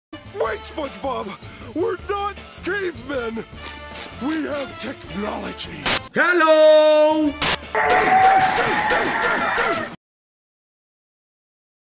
torres spongebob
mr torres loves spongebob so much he wanted to say hi!!

after effects is hard to use on a trash laptop